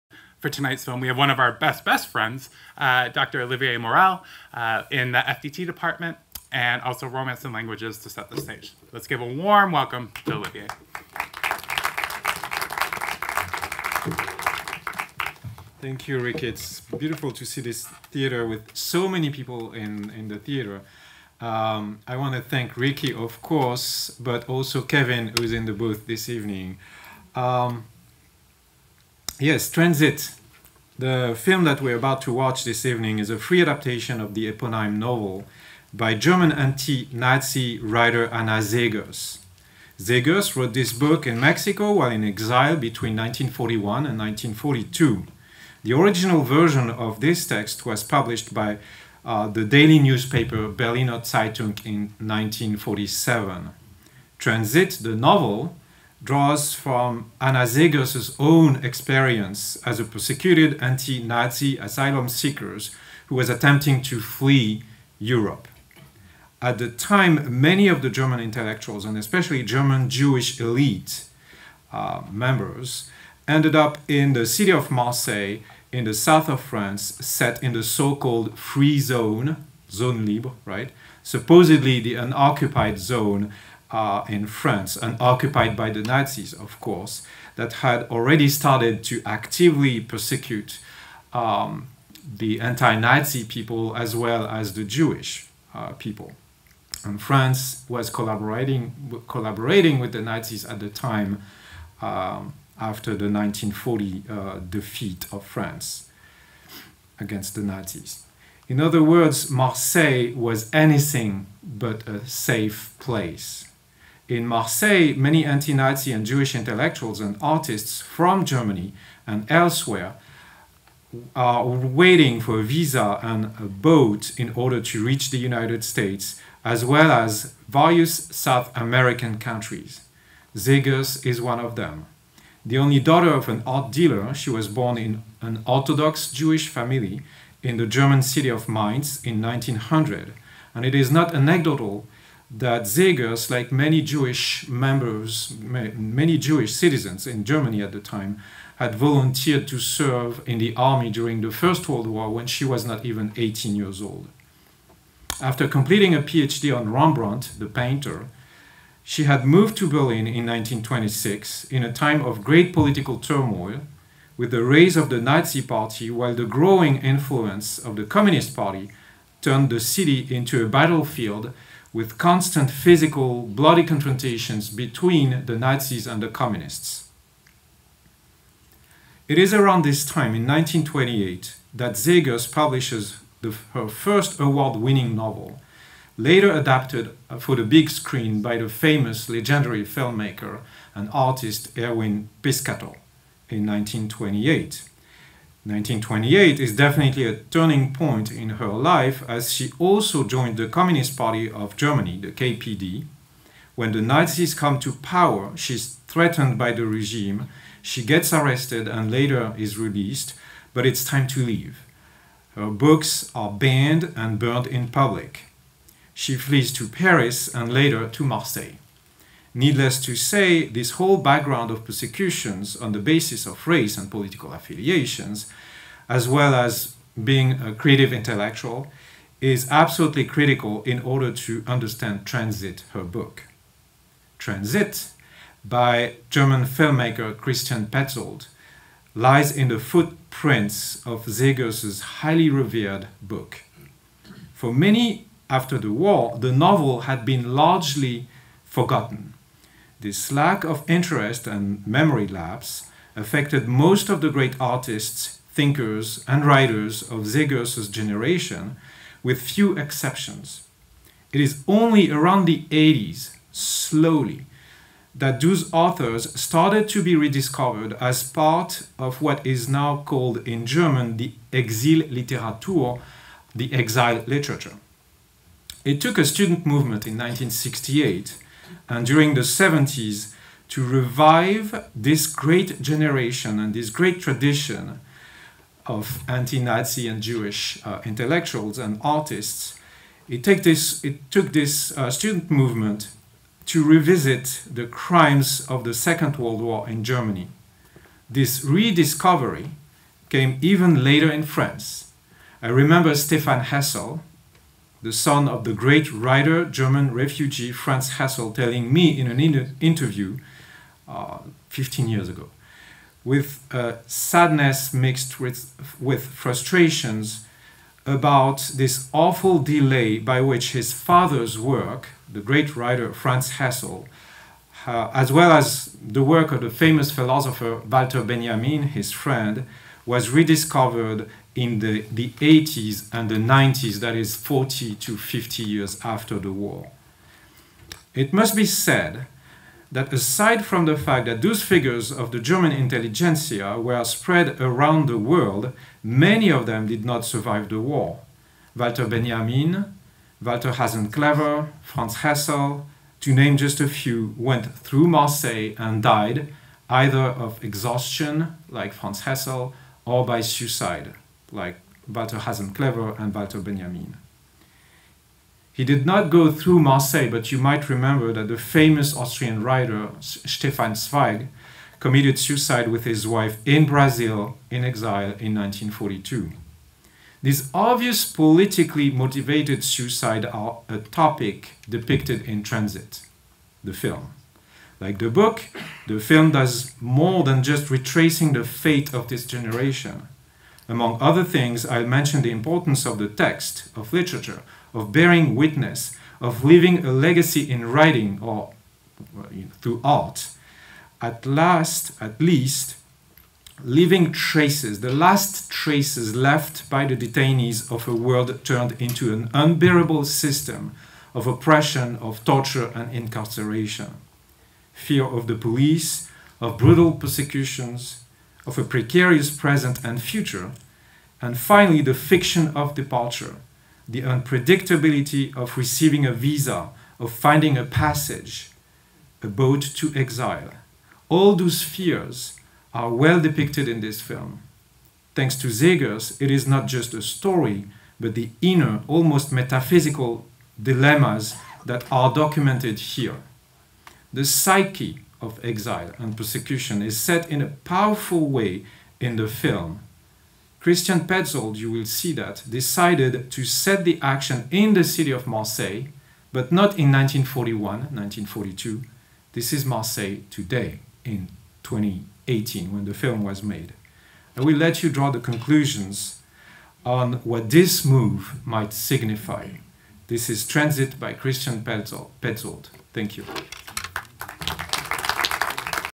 Film Introduction: Transit
Directed by Christian Petzold
April 25, 2019
Browning Cinema of the DeBartolo Performing Arts Center